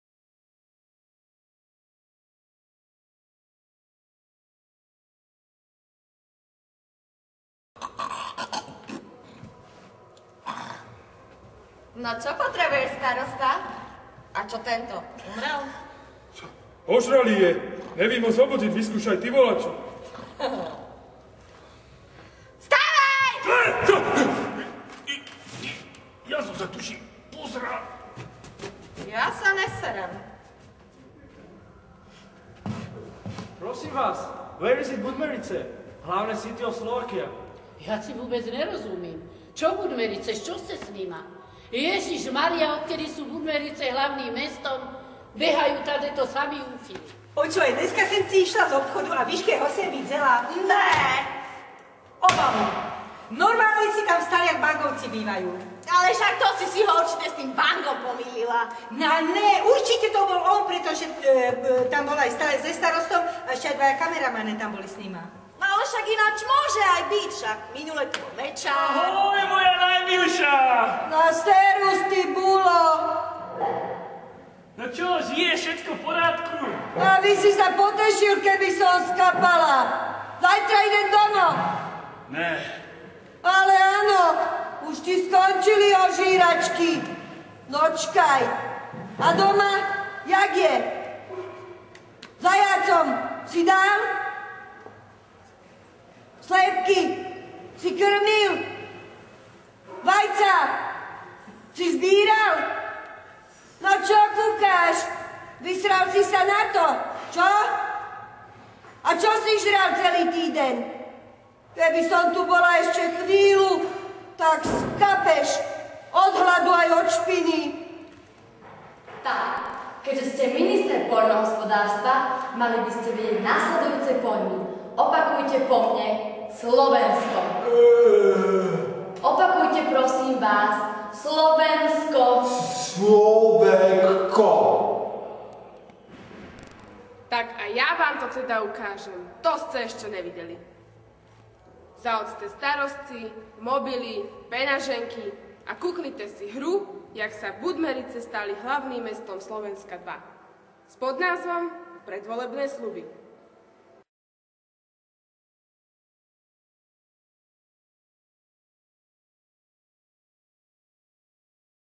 Jak sa Budmerice stali hlavným mestom Slovenska II - trailer
Pozývame Vás na premiéru hry Budmerického divadla - Jak sa Budmerice stali hlavným mestom Slovenska II - predvolebné sľuby